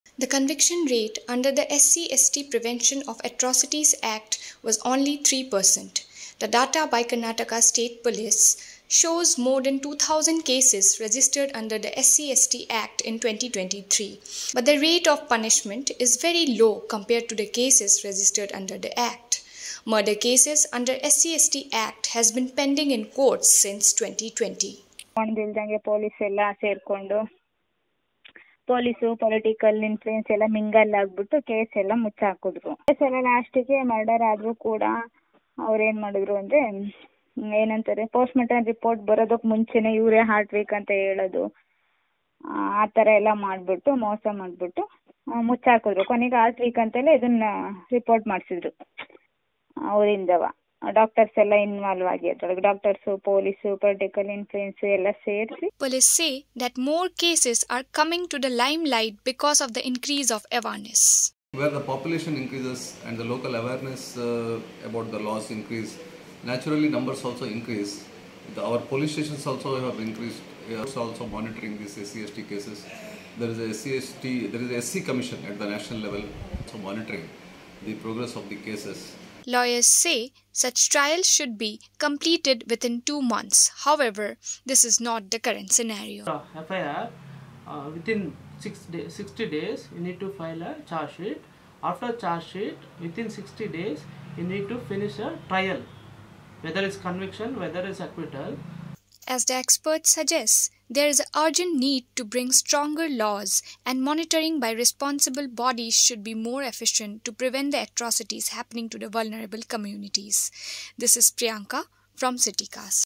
Low conviction rate for crimes booked under SC/St Act
Reported by Priyanka Saharia
As Karnataka sees a rise in crimes against SC/ST, compared to the cases conviction rate is very low. Most of the cases are pending in the courts.